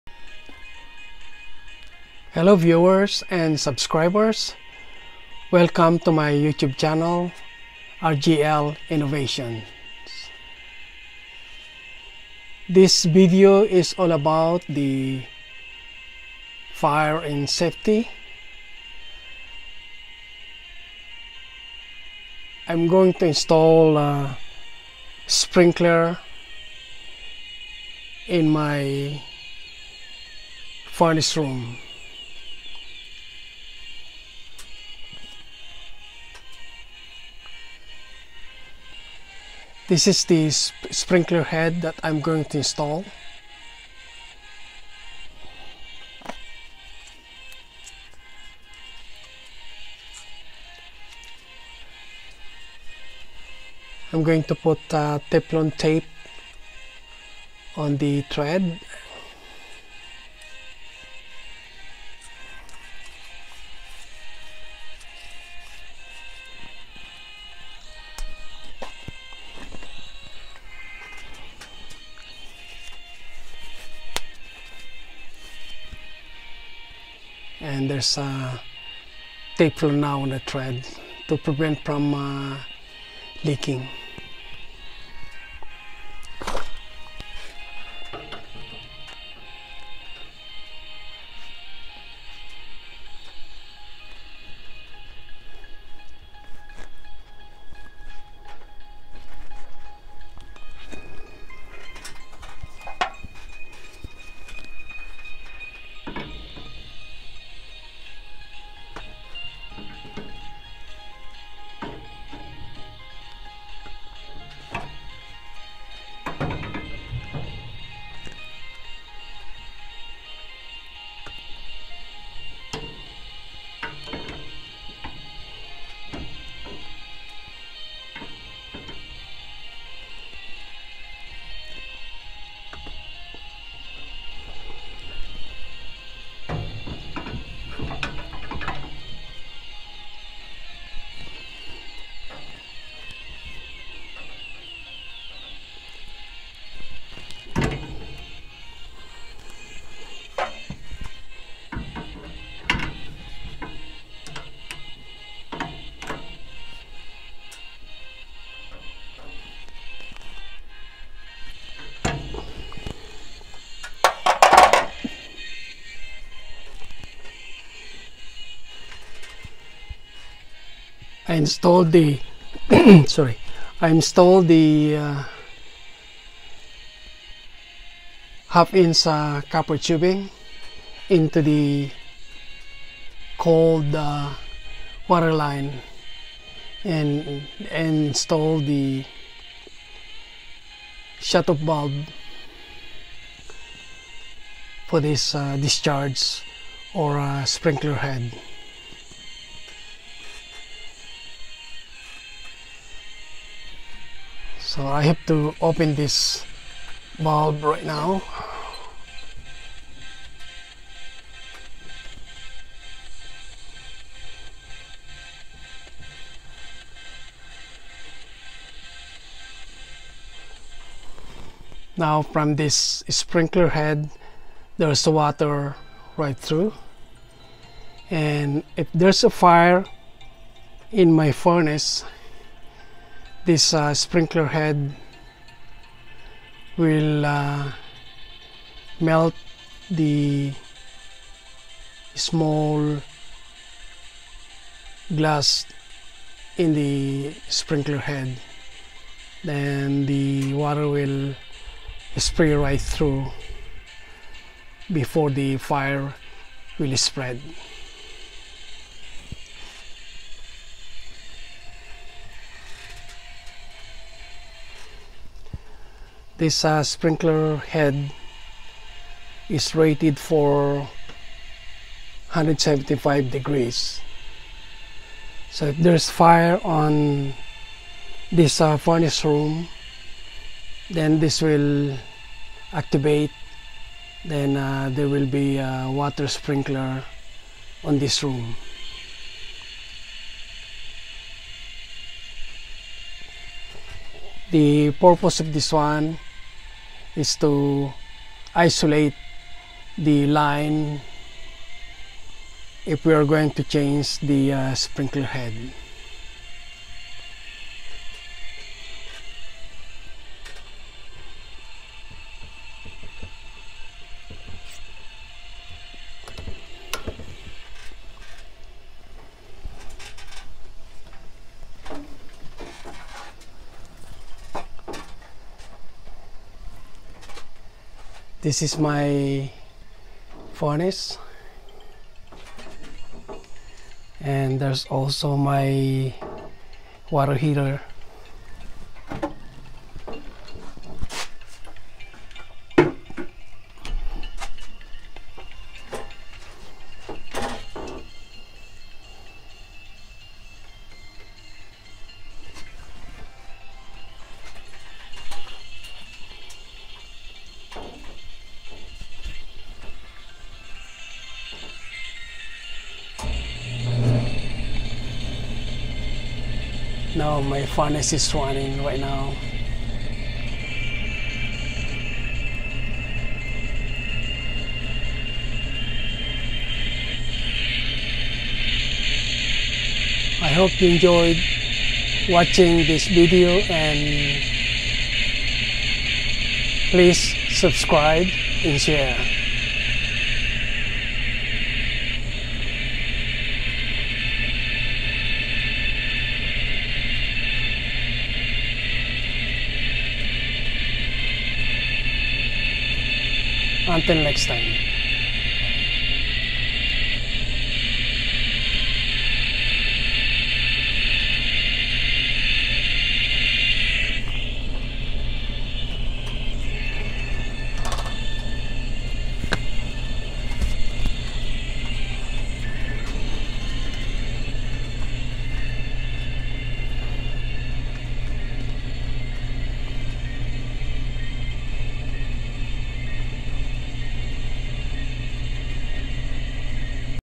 DIY How to install fire sprinkler in the Furnace room
Installed fire sprinkler in the Furnace room. This is for fire and safety hazard. If there is a fire in the Furnace room, this fire sprinkler will activate. There is a small glass in the middle of sprinkler head rated for 175 F. When melted the water will spray before the fire will spread. The 1/2” copper tubing was hooked up in the cold water line. Installed water valve for maintenance purposes like replacing defective or leaking sprinkler head.